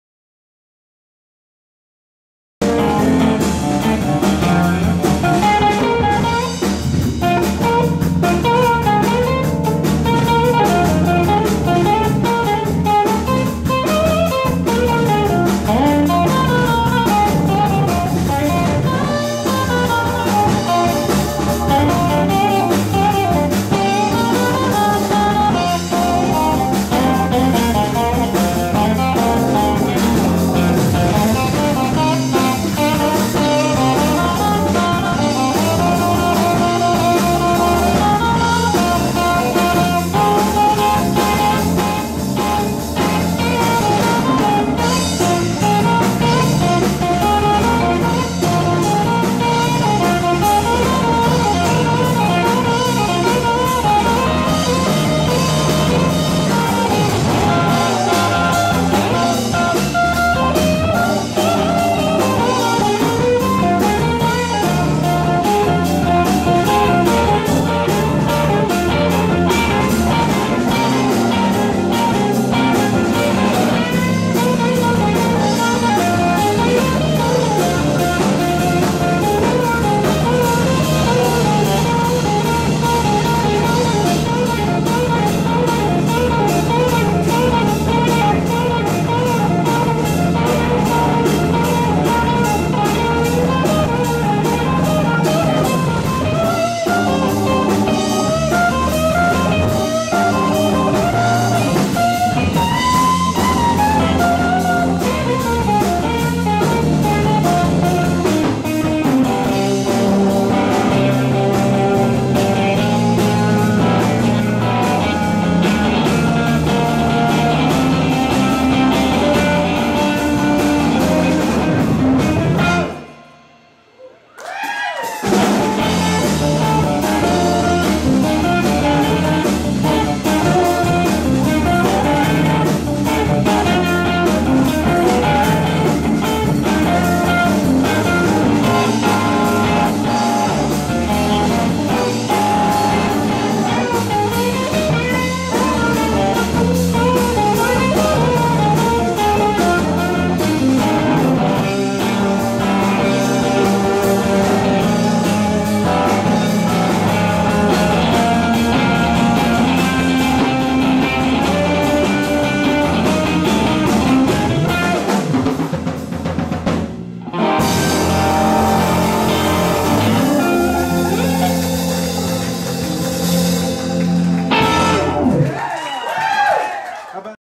Laura Chavez at Murphy's Law in Sunnyvale CA
Laura Chavez, Jack Cannon  and Baldhead Fred Rautmann tore up a short set at Murphy's law on Sept. 2009.  Any time we get Laura back in the Bay area is a good day!

It was great to see the band back together.